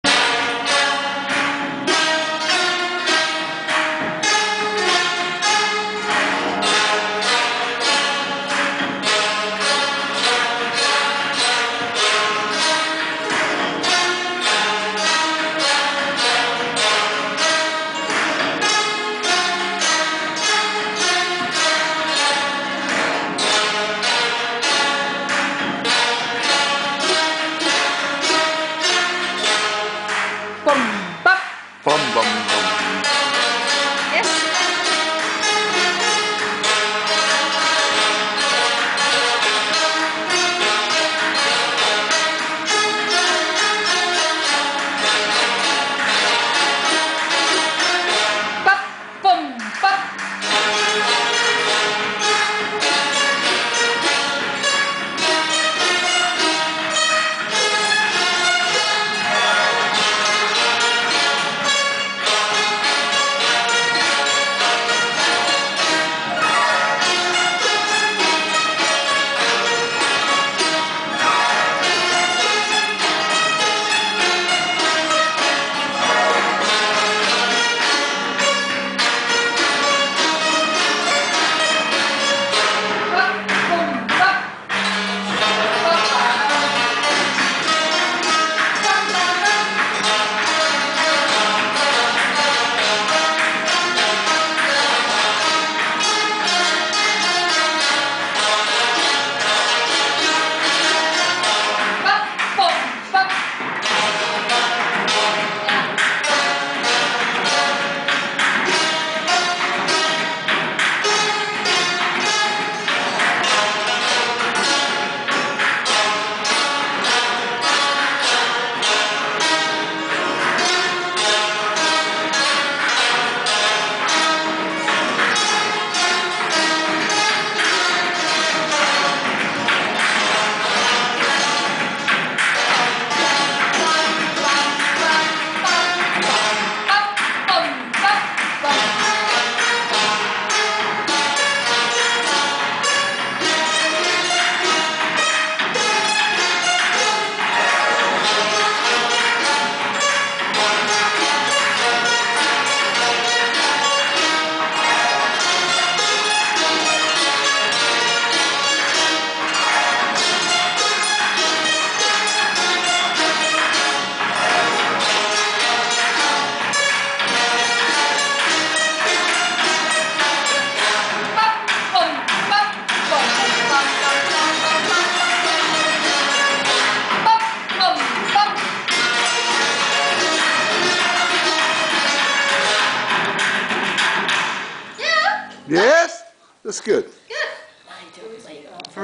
Rehearsal of Vietnamese Zither Ensemble, OTTAWA, CANADA
Rehearsal of the Vietnamese Zither Ensemble conducted by Hiền Minh, Thuận, and Vi Vân in Ottawa, Ontario, CANADA before the show at Museum of Civilizations in Gatineau on May 9th 2009

Filmed by Tran Quang Hai, May 8th 2009